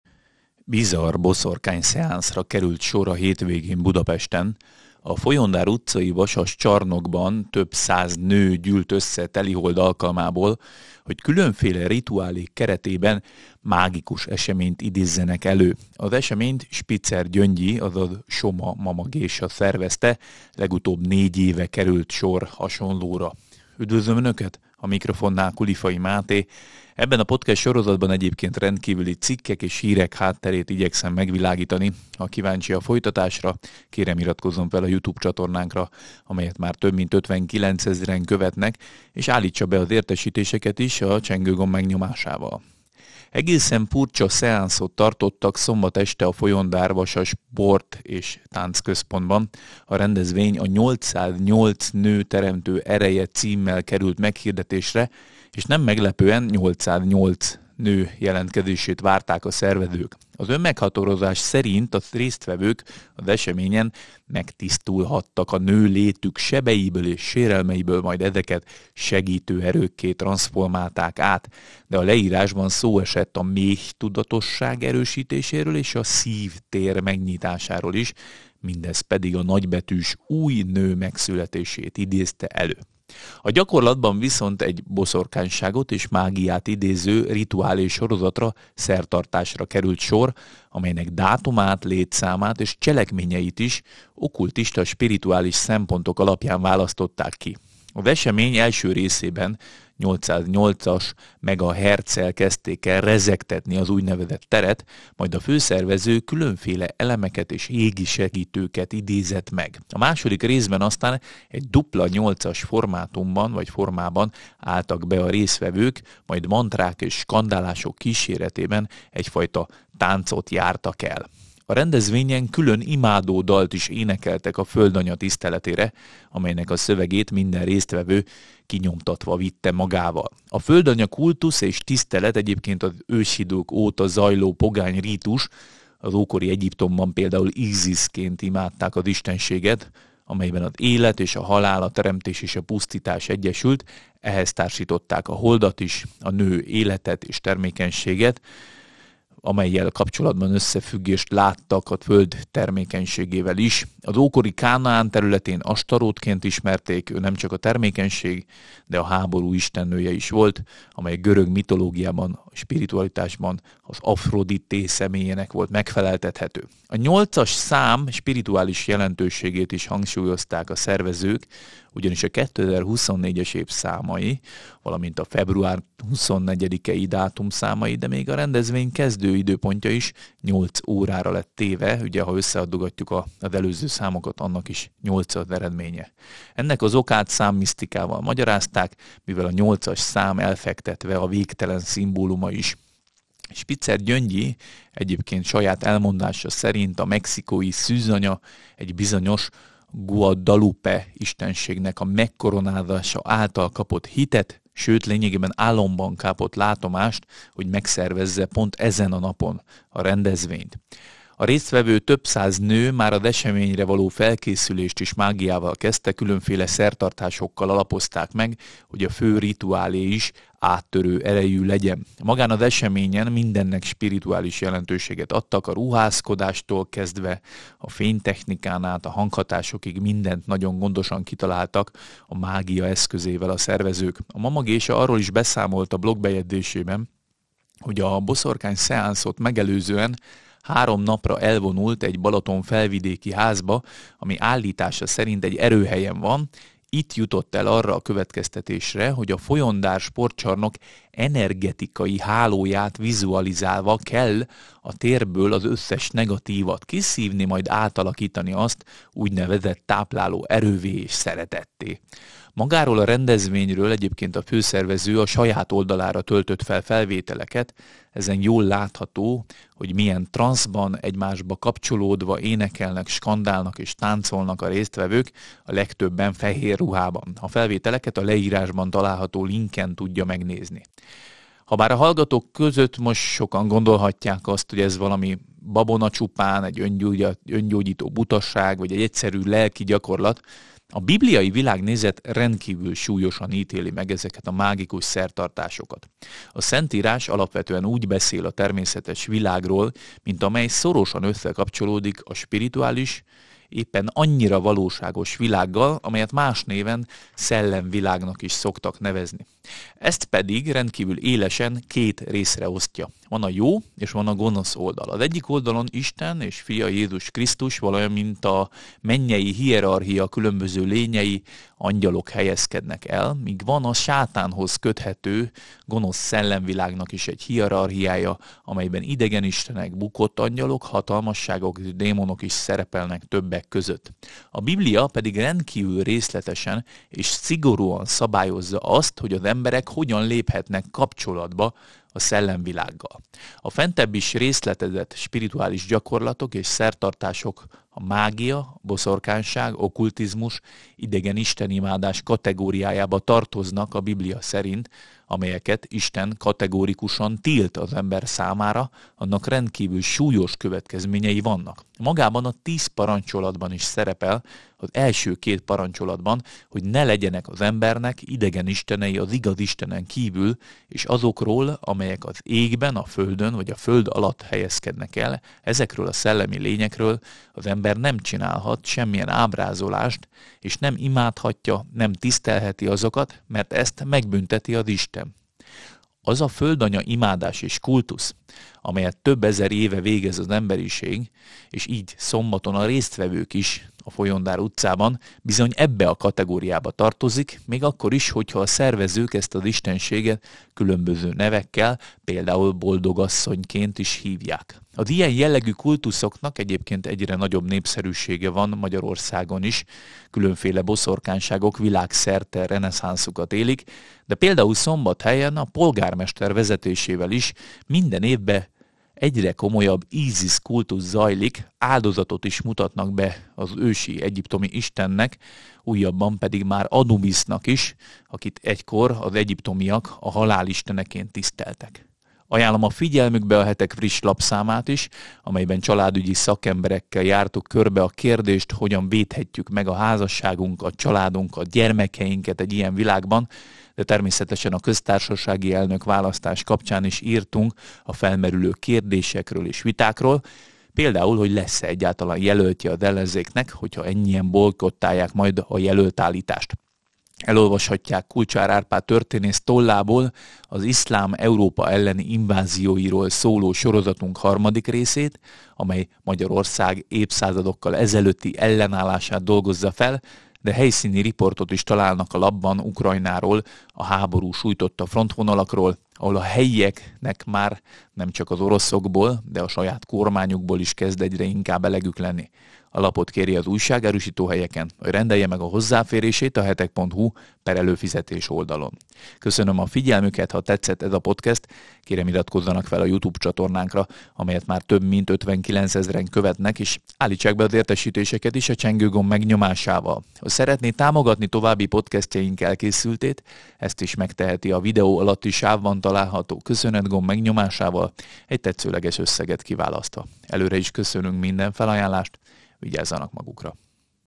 Bizarr boszorkányszeánsz Budapesten: összegyűltek a mágusok | Rendkívüli hírek
Bizarr boszorkányszeánszra került sor a hétvégén Budapesten. A Folyondár utcai Vasas-csarnokban több száz nő gyűlt össze telihold alkalmából, hogy különféle rituálék keretében mágikus eseményt idézzenek elő. Az eseményt Spitzer Gyöngyi, azaz Soma Mamagésa szervezte, legutóbb 4 éve került sor hasonlóra.

Képforrás: Facebook/Spitzer Gyöngyi//Shutterstock/Puzzlepix

Exkluzív tartalmakért kérjük csatlakozzon a Hetek Podcast Támogatói Körhöz